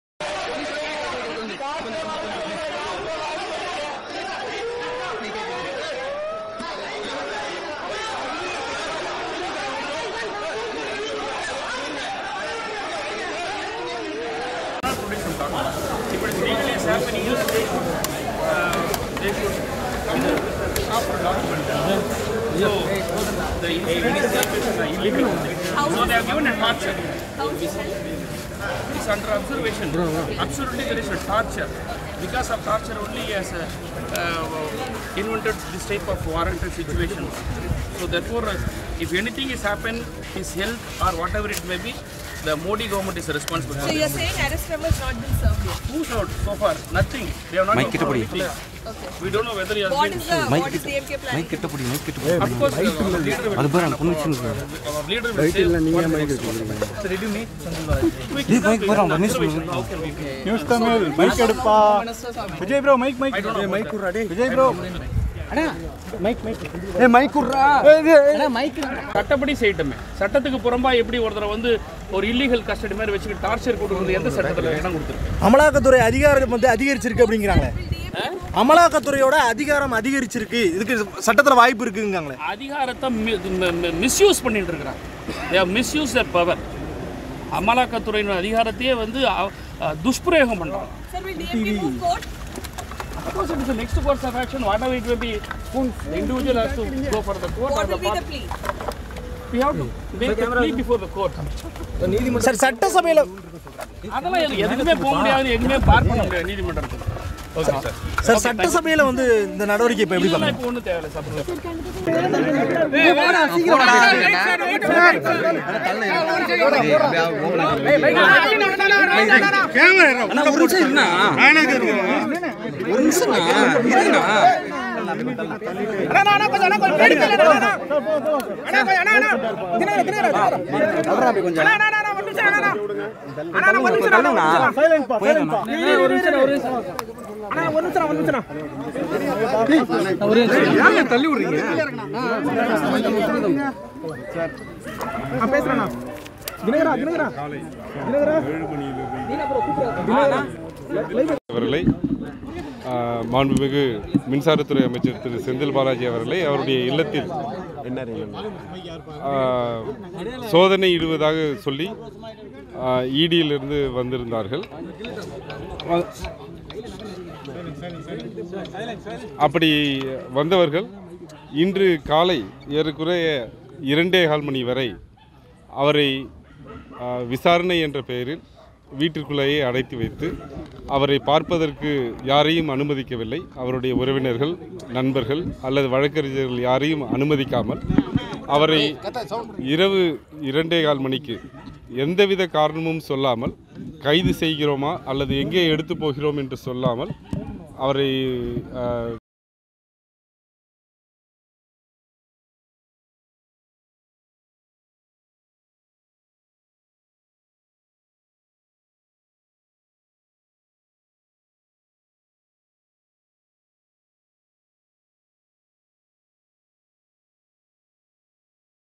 🔴BREAKING: DMK's Senthil Balaji Arrested By ED in Money Laundering Case | செந்தில் பாலாஜி கைது
In this video, we bring you the latest news on the arrest of DMK Minister Senthil Balaji by the Enforcement Department in a high-profile money laundering case. Stay updated with the unfolding events as we delve into the details of this significant development. Our team provides comprehensive coverage, analysis, and insights into the political landscape, shedding light on the impact of such cases on the state's administration.